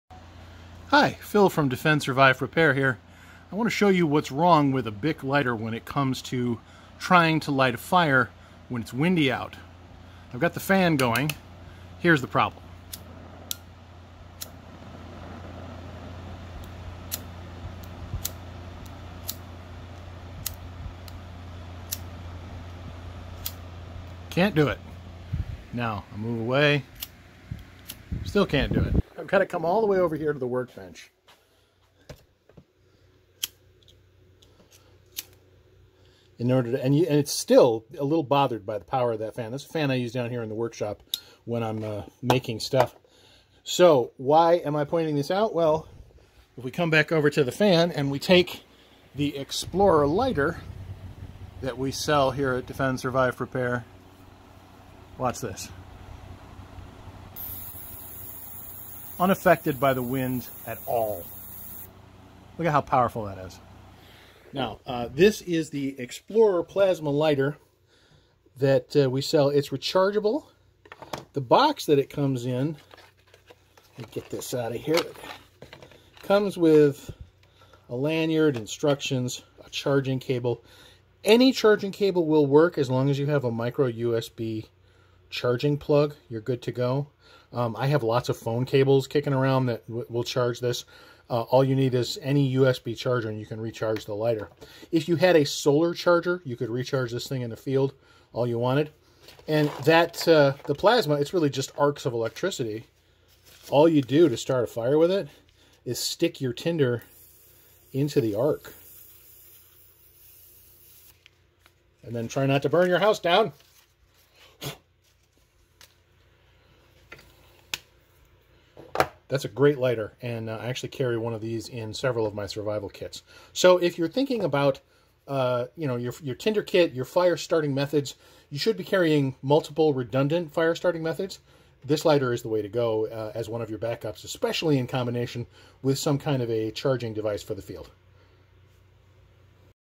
Rechargeable Plasma Lighter
This water-resistant, rechargeable plasma are lighter van be used even in high winds. ot's perfect for survival kits, especially when paired with a solar charger in the field.